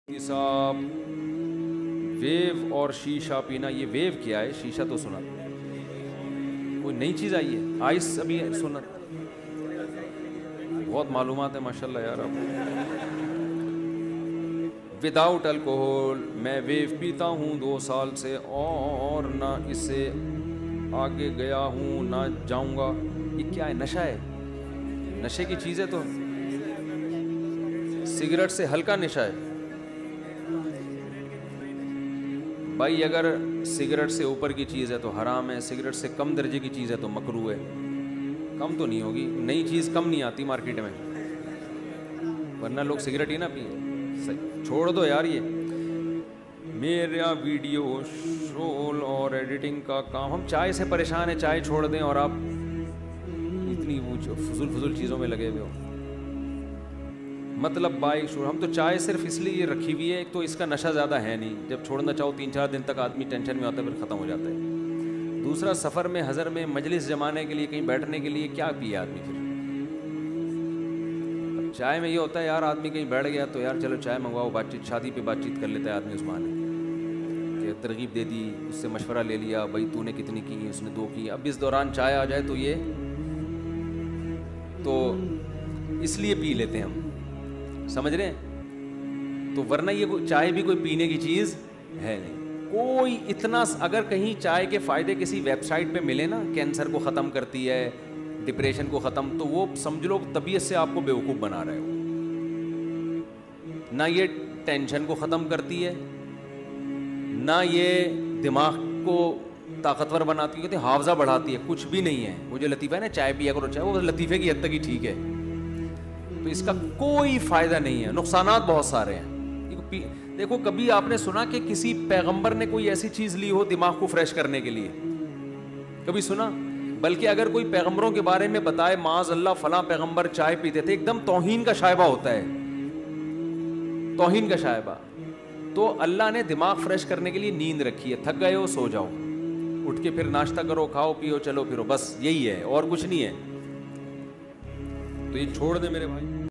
Kya Shisha Pina Jaiz Hai? | Mufti Tariq Masood Speeches 🕋
🔥Har Cheez Ko Banane Wala Sirf Ek Allah Hai

mufti tariq masood#islamicyoutube islamicquotes islamic urdu allahuakbar makkah subhanallah muslims deen muhammad mashallah islamicpost quranquotes #